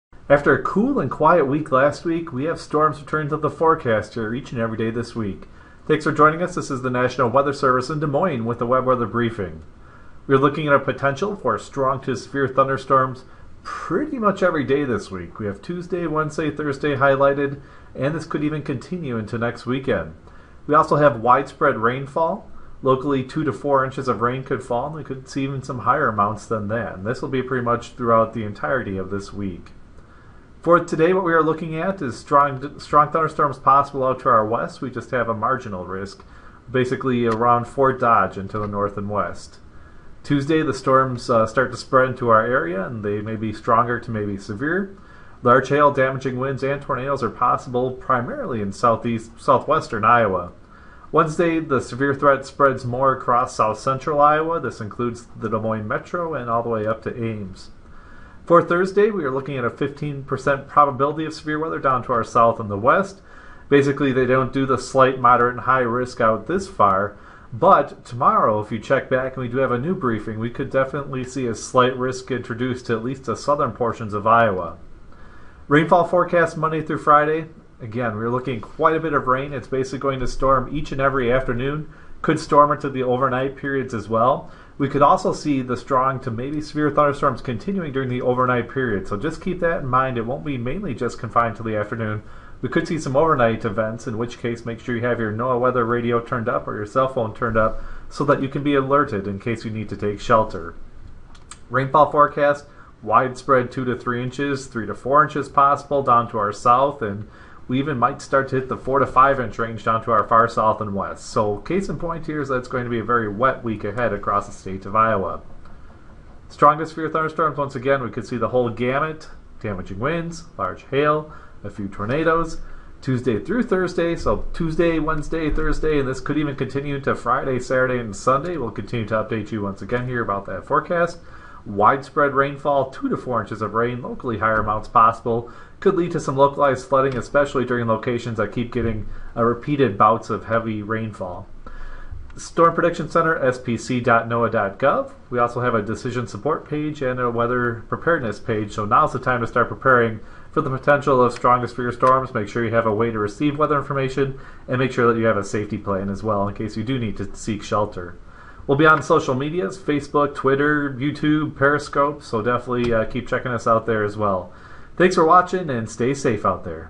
NWS Des Moines Weather Briefing May 23, 2016
Strong to severe storms, along with heavy rainfall, is possible this week across Iowa. Watch this web weather briefing for your latest forecast details.